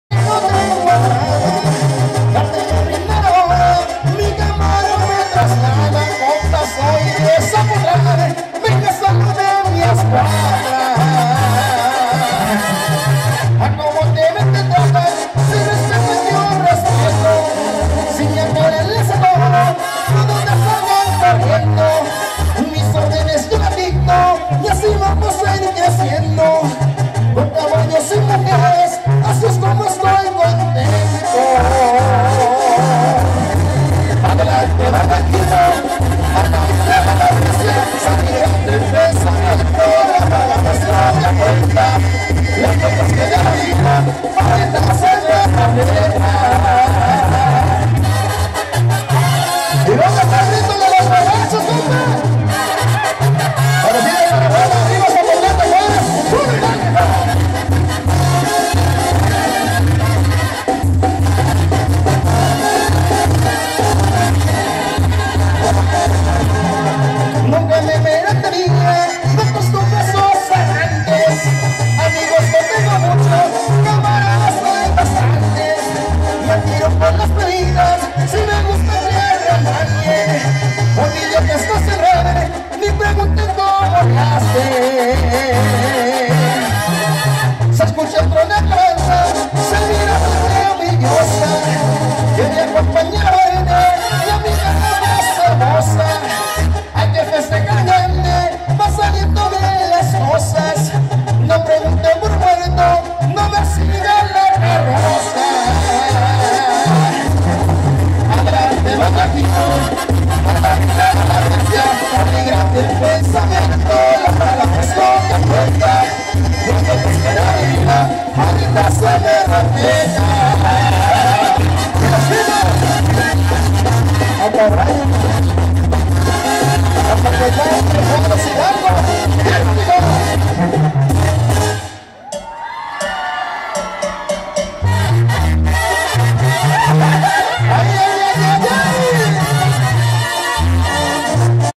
Fiesta patronal Zapotlan de Juarez Hgo 2011
Tradicional fiesta del 8 de Diciembre del 2011 en Zapotlan de Juarez  con la Banda San Jose de Mesillas